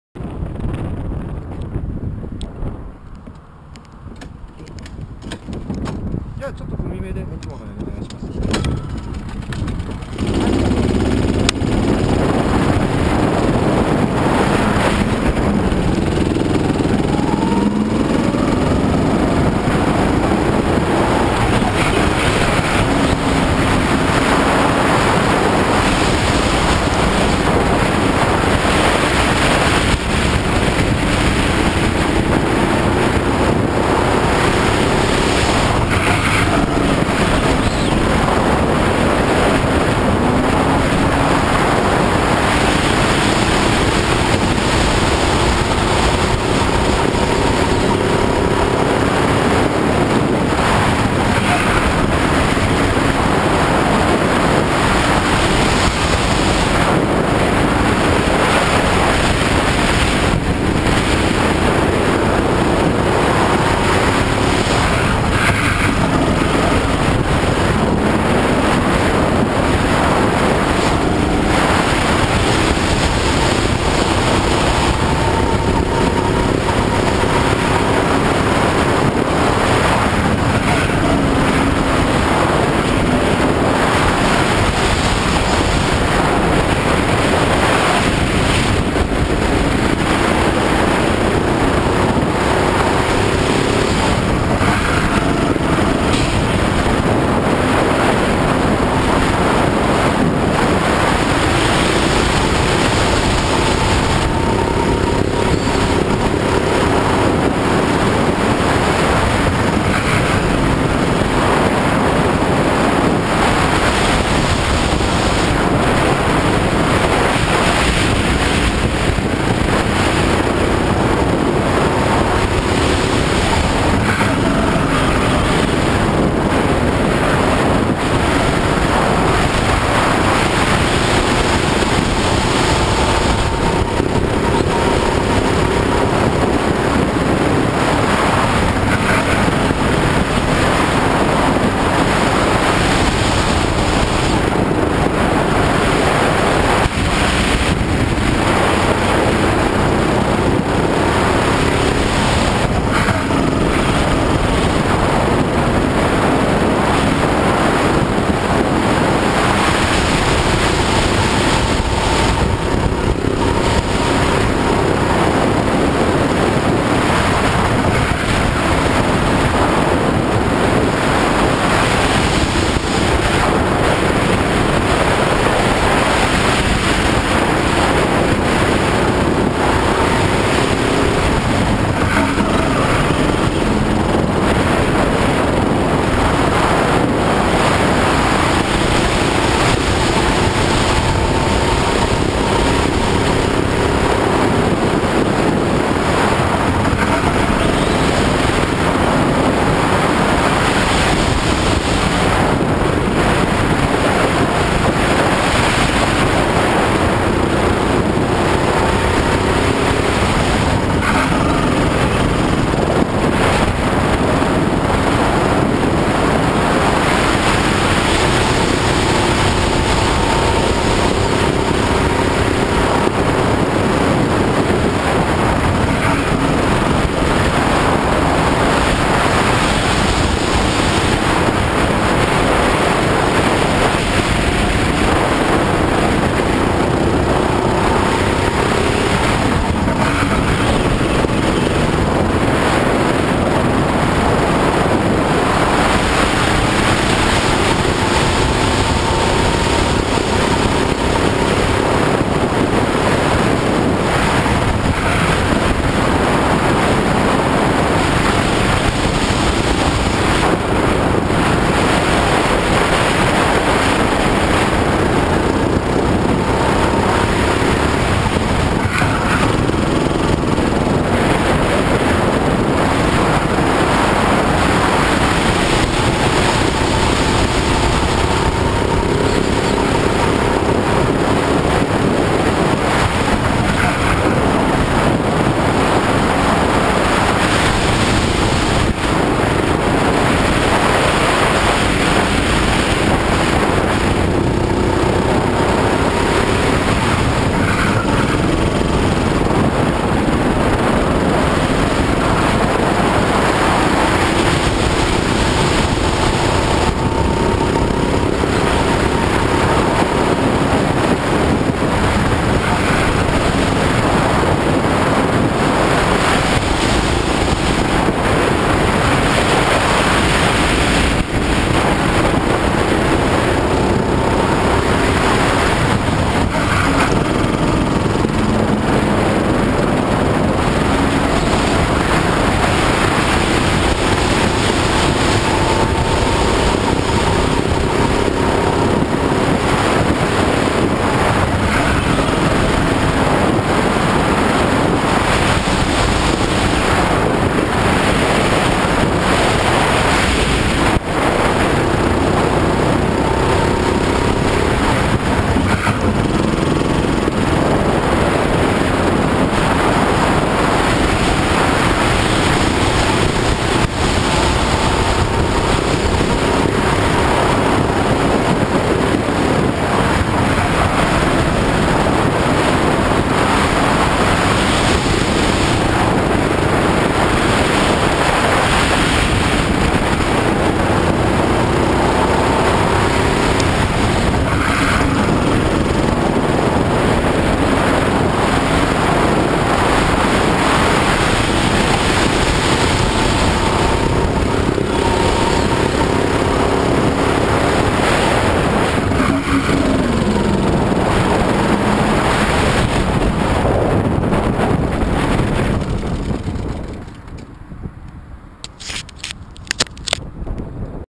Free Practice of Reverse Drive Act.3 @SSGP on 20140524
湘南シーサイドグランプリ、スタンダードコース（ショート）逆走3本目。
ほぼ全域でMZ特有の息継ぎ症状が出るので、タイムを安定させるのが結構しんどいです。